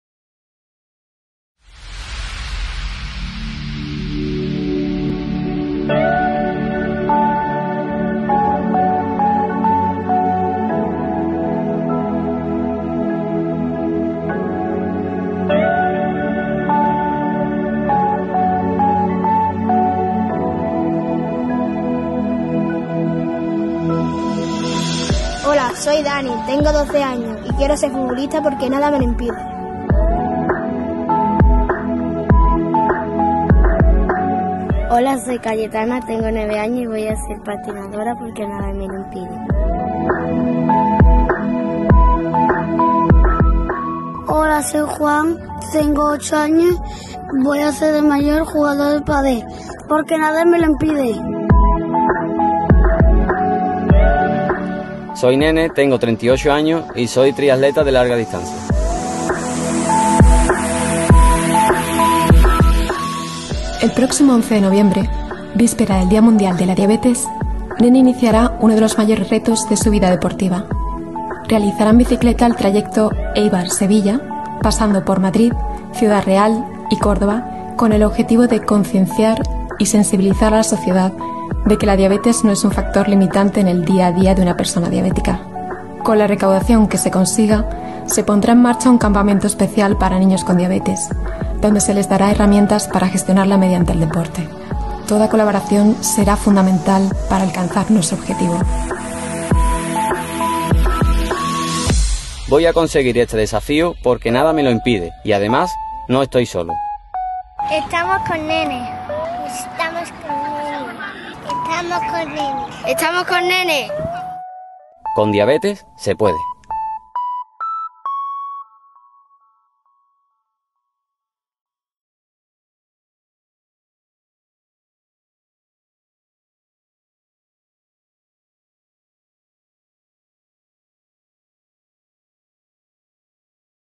Ride 4 Children Diabetes Team
Hoy se ha presentado en Quirónsalud Sagrado Corazón el Desafío Éibar-Sevilla 72 horas con diabetes Tipo1 que realizará el deportista sevillano "Nene", todo un ejemplo de superación, sacrificio y tesón para demostrar que con Diabetes Tipo1 puedes hacer todo lo que te propongas. Los fondos recaudados irán destinados a los campamentos de niños diabéticos de la Asociación de Diabéticos Anadis.